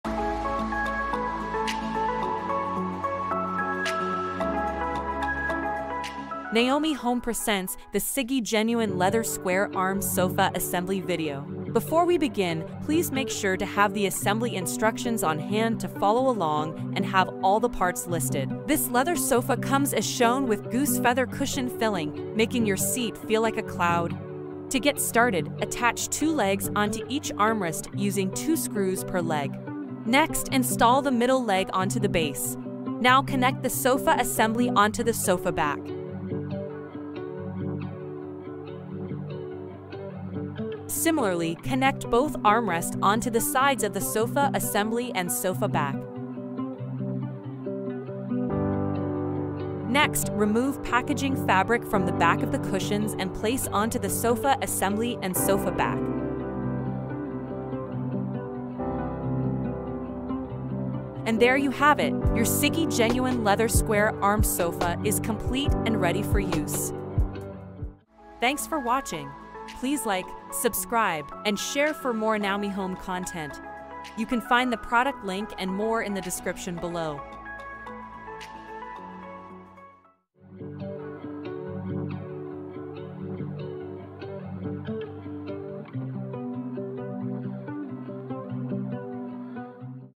How to Style Your Living Room: Naomi Home | Siggy Sofa Assembly Video
Naomi Home | Siggy Sofa Assembly Video.
Adorn your living room with our glamourous genuine leather sofa. The Naomi Home Siggy Sofa is the first thing that will help you wind down after a long day at work. Its comfortable cushion is composed of goose feather cushion filling, making your seat feel like a cloud. You will look forward to enjoying your down time in its roomy, deep seats and ideal backrest.

Intro & Product Intro: 0:00 - 0:11
Sofa Pieces Guide: 0:12 - 0:24
Sofa Assembly: 0:25 - 0:59
Outro: 1:00 - 1:31